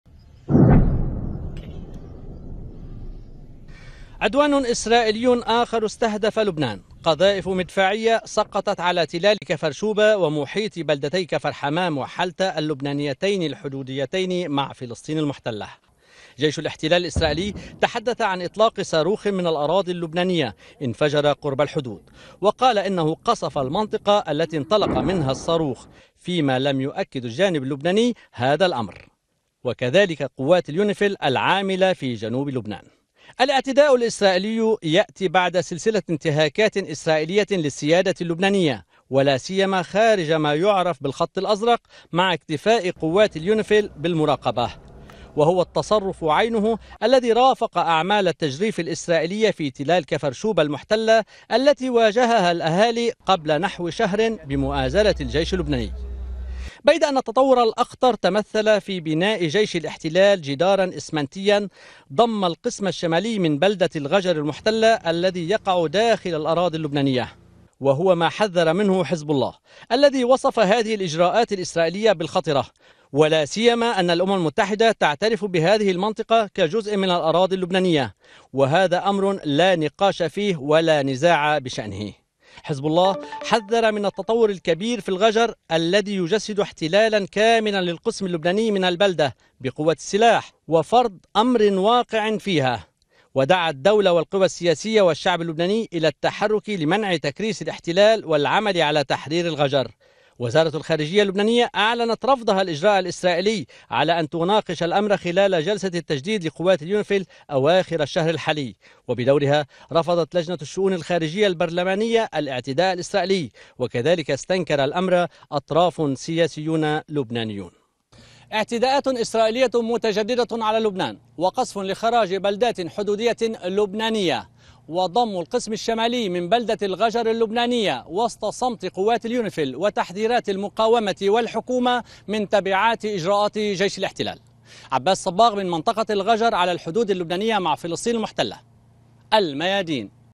قصف إسرائيلي يرفع منسوب التوتر على الحدود اللبنانية
اعتداءات اسرائيلية جديدة تستهدف لبنان حيث سجل الجيش اللبناني سقوط نحو خمس عشرة قذيفة مدفعية على خراج بلدات كفرشوبا وكفرحمام وحلتا الحدودية, وتزامن ذلك مع اعتداء إسرائيلي آخر تمثل بإنشاء سياج شائك وبناء جدار أسمنتي حول كامل بلدة الغجر المحتلة ما يفصلها عن محيطها ويكرسها ضمن الاراضي المحتلة على نحو ينزع عنها هويتها اللبنانية.

إعداد: عباس الصباغ